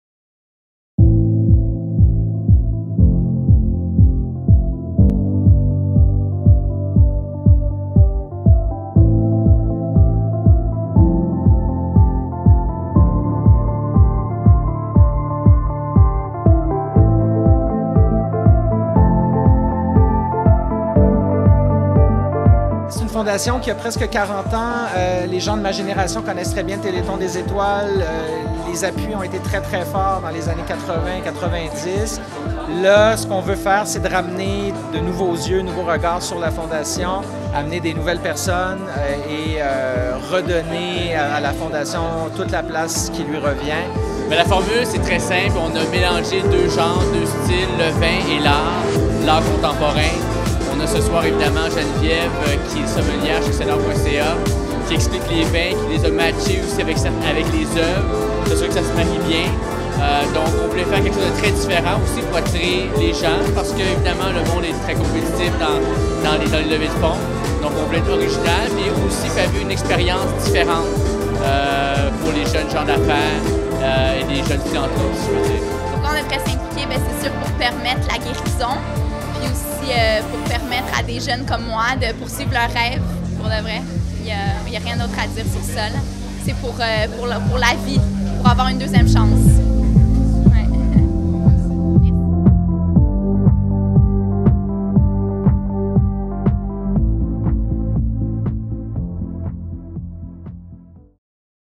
1er Pop-up Art & Vin
Cet événement avait pour but de permettre aux jeunes professionnels d’échanger sur l'entrepreneuriat, de créer des opportunités d’affaires et surtout d’amasser des fonds pour financer la recherche sur les maladies infantiles au Québec.

Sous la coprésidence de Monsieur Vy Nguyen (Banque UBS Canada), Madame Catherine Dupéré (BDC) et Madame Sandra Ferreira (Groupe Ferreira), les invités ont pu profiter d’une dégustation de vin commentée par la sommelière Geneviève Boucher (CellArt.ca), également coprésidente. Le tout était accompagné de délicieux mets gastronomiques offerts par Taverne F. C'est au son de Vincent Lemieux, DJ et programmateur à MUTEK Montreal co-fondateur du label Musique Risquée ainsi que producteur, que les invités ont pu admirer les œuvres de la Galerie d'art urbain Station 16.

Un grand merci à tous nos invités, partenaires et bénévoles! Grâce à vous, la recherche sur les maladies infantiles progresse pour que davantage d’enfants malades puissent guérir!

Crédit vidéo : Republik x ABCDF

This event aimed to enable young professionals to exchange ideas on entrepreneurship, create business opportunities, and especially to raise money to fund research on childhood diseases in Quebec.

Co-chaired by Mr. Vy Nguyen (UBS Canada Bank), Mrs. Catherine Dupéré (BDC) and Mrs. Sandra Ferreira (Groupe Ferreira) Guests enjoyed a wine tasting session guided by sommelier Geneviève Boucher (CellArt.ca), also Co-Chair of the evening. This was accompanied by delicious gourmet food offered by Taverne F. Vincent Lemieux, DJ and MUTEK programmer, co-founder of the Musique Risquée label and producer, delighted the music lovers amongst the guests. For the art lovers, the Urban Art Galerie Station 16 on Saint-Laurent was the place to be.

A very big thank you to all our guests, partners and volunteers! Thanks to you, research on childhood diseases progresses and many more sick children will benefit from it!

Video credit : Republik x ABCDF